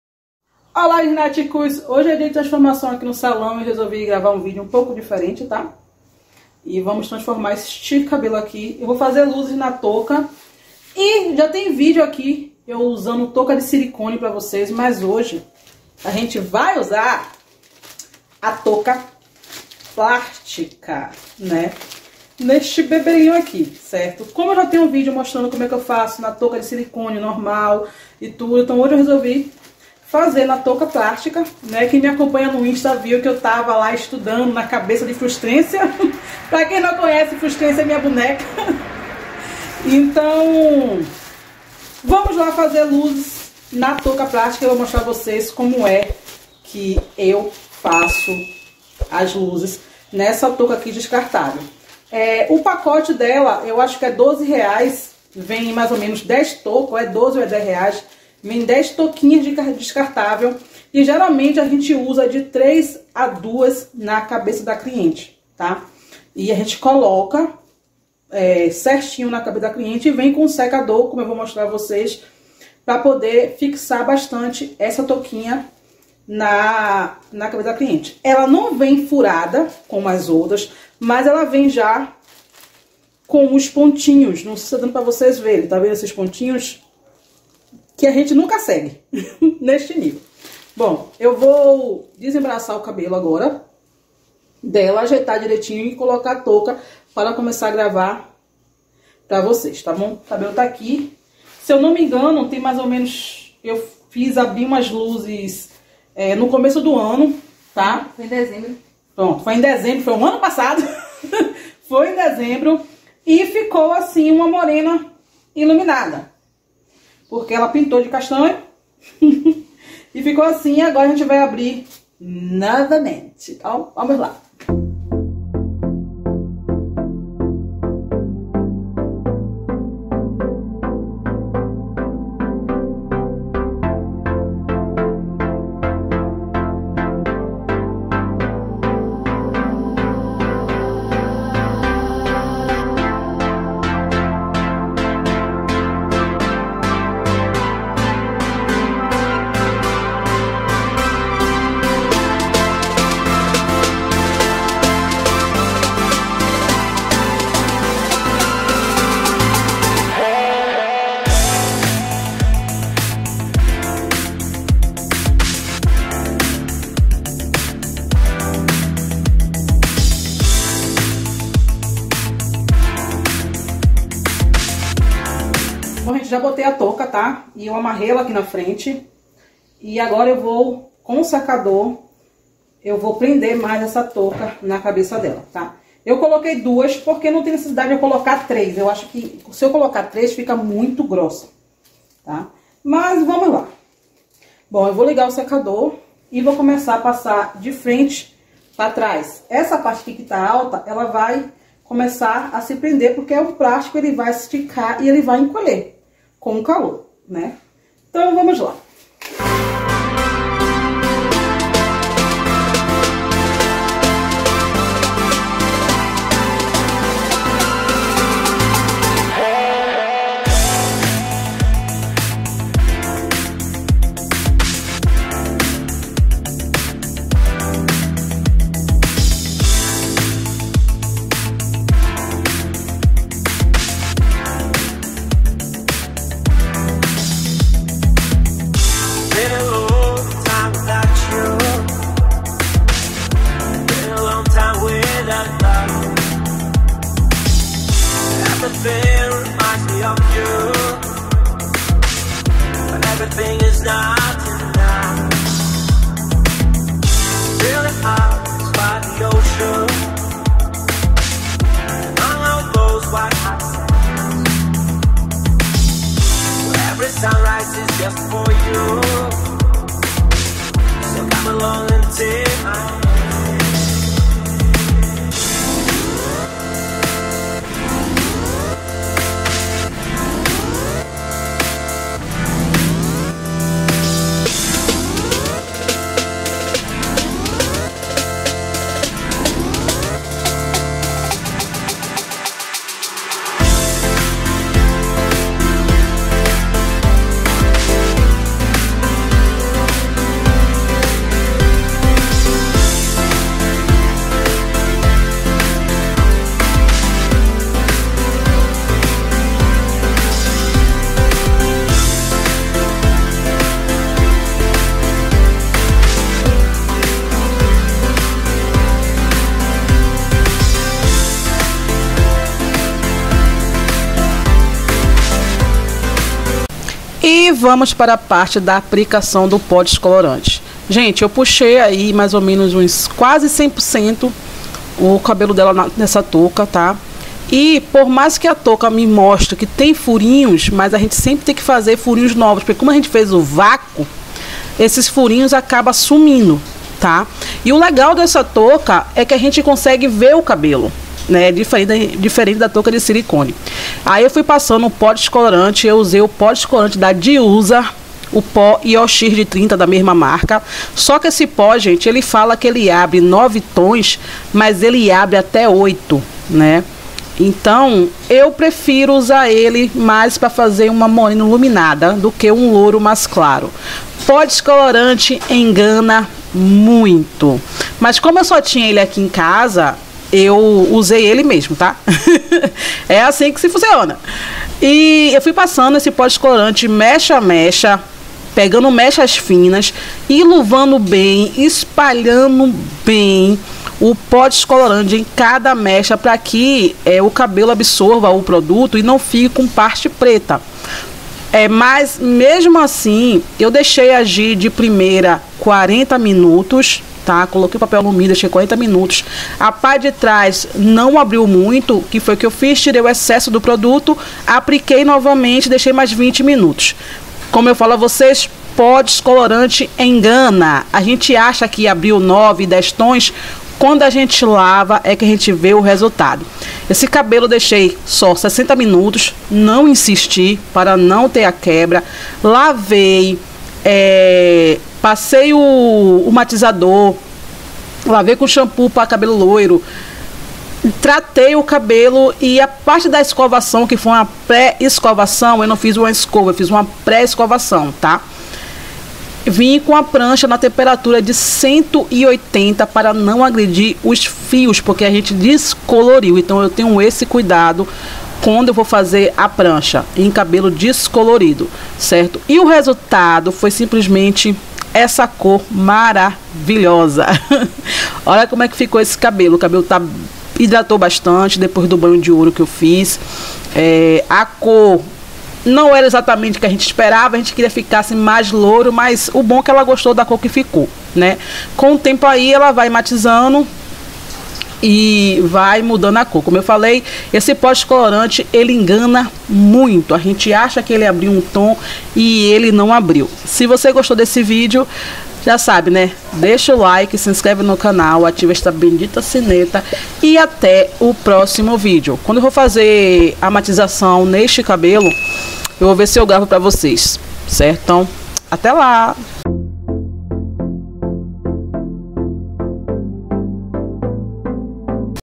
MECHAS NA TOUCA DESCARTAVÉL / TRANSFORMAÇÃO
▬▬▬▬▬▬▬▬▬▬▬ஜ۩۞۩ஜ▬▬▬▬▬▬▬▬▬▬▬▬▬
Olá meus amores, no vídeo de hoje vamos fazer mechas em touca descartavel, eu particulamenteamo fazer luzes na touca.